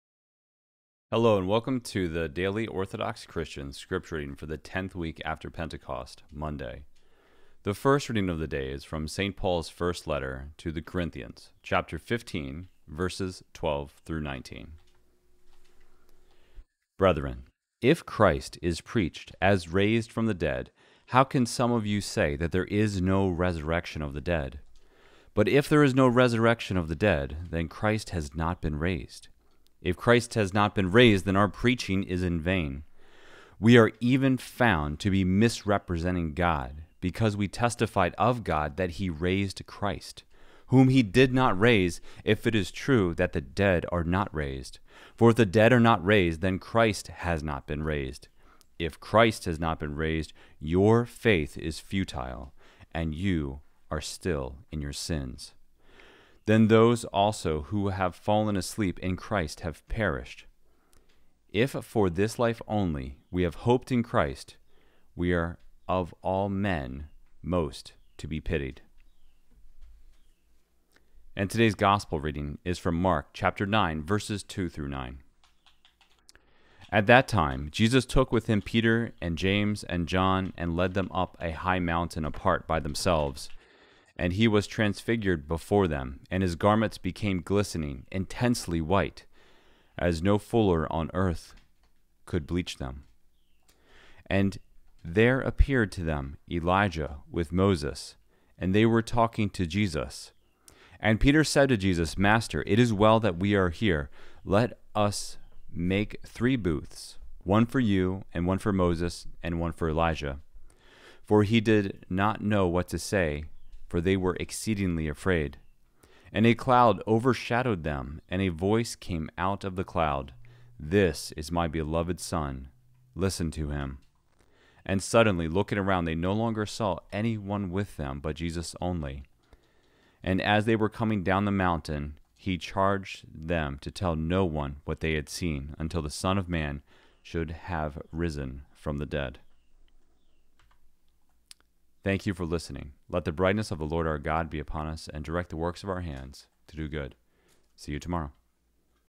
2023-08-07: Daily Orthodox Christian Scripture Reading - 1 Cor 15:12-19, & Mark 9:2-9
Daily Orthodox Christian Scripture Reading for the Tenth Week after Pentecost: Monday

ST. PAUL'S FIRST LETTER TO THE CORINTHIANS 15:12-19

MARK 9:2-9